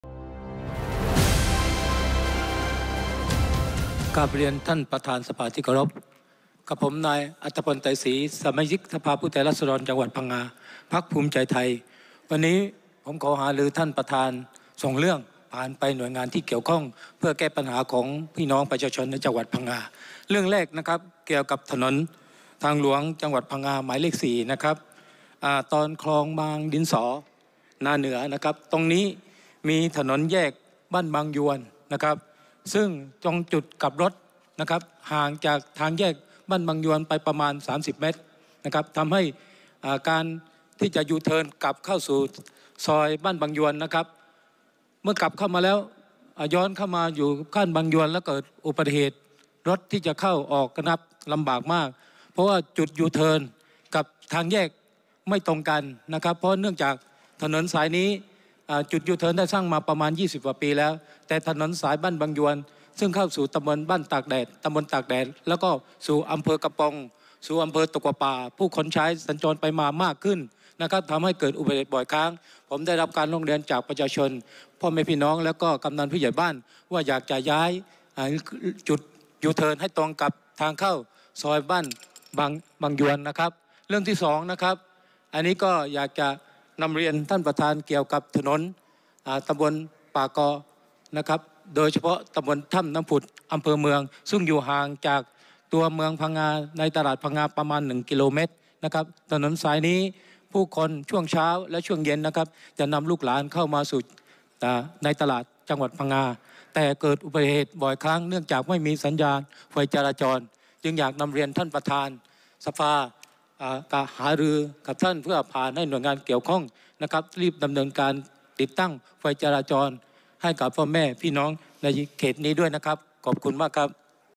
“อรรถพล” นำปัญหา ย้ายจุดยูเทิร์น และไฟจราจร หารือเร่งดำเนินการแก้ไข เพื่อ ปชช. จ. พังงา
วันที่ 20 ธันวาคม 2566 นายอรรถพล ไตรศรี  สมาชิกสภาผู้แทนราษฎรจังหวัดพังงา เขต 2 พรรคภูมิใจไทย ได้นำปัญหาของประชาชนหารือ ในการประชุมสภาผู้แทนราษฎร ครั้งที่ 3  ได้มีการเร่งรัด กรมทางหลวง ย้ายจุดยูเทิร์นบนถนนทางหลวงจังหวัดหมายเลข 4 ตอนคลองบางดินสอนาเหนือ ที่อยู่ห่างจากจุดแยกบ้านบางยวน เนื่องจากจุดยูเทิร์นกับทางแยกไม่ตรงกัน ทำให้เกิดอุบัติเหตุบ่อยครั้ง

ทั้งนี้ได้มีการย้ำถึงการเร่งแขวงทางหลวงพังงา ให้ติดตั้งสัญญาณไฟจราจรบนถนนสายบ้านป่ากอ ต.ถ้ำน้ำผุด อ.เมืองพังงา เนื่องจากเป็นเส้นทางที่ผู้คนใช้สัญจรไปมาเป็นจำนวนมากในช่วงเช้าและเย็น ทำให้เกิดอุบัติเหตุบ่อยครั้ง

สส.อรรถพล ไตรศรี
สมาชิกสภาผู้แทนราษฎรจังหวัดพังงา เขต 2
พรรคภูมิใจไทย